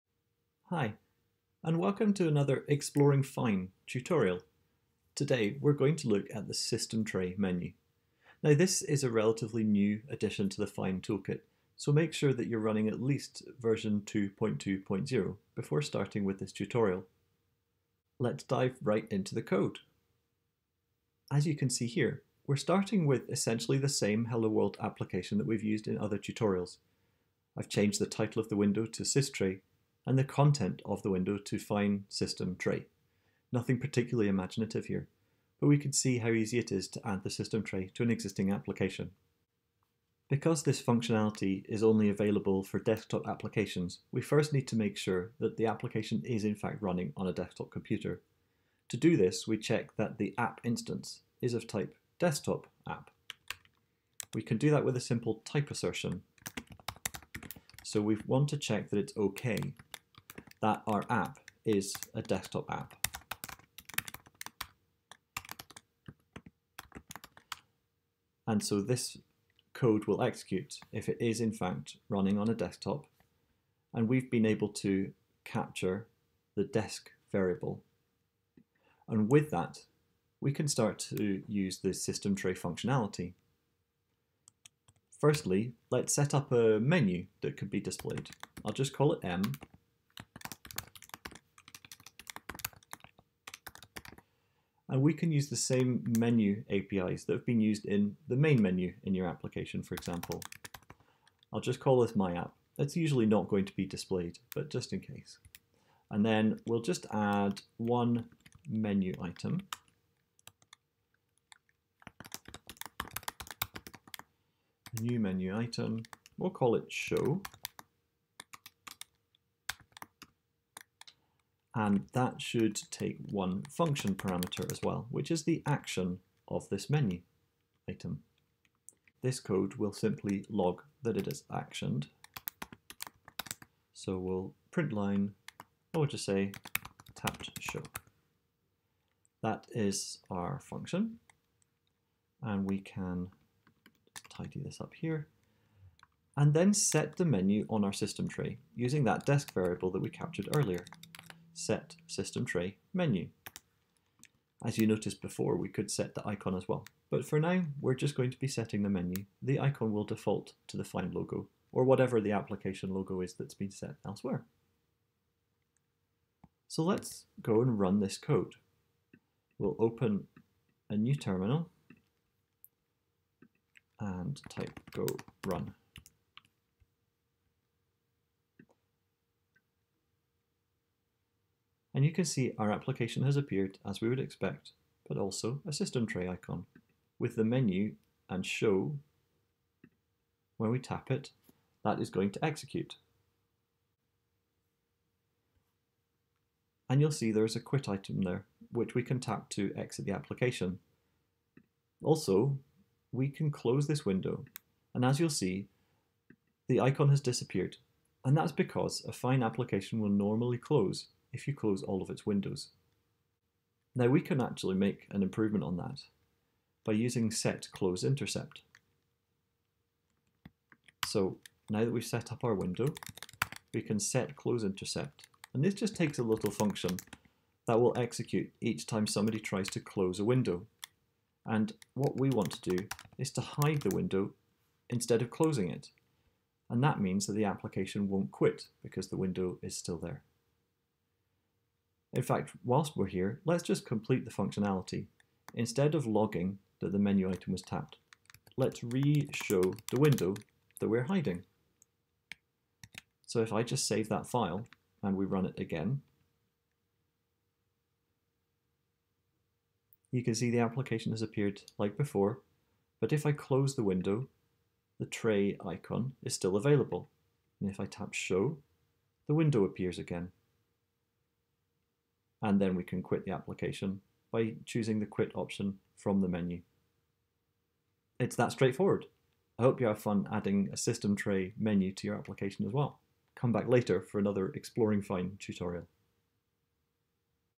Exploring Fyne: System Tray
Since Fyne v2.2.0 it has been possible to add a menu to the system tray (in macOS, Windows and Linux) in this tutorial we see how to do that.
Step by step to go from hello world to hello systray!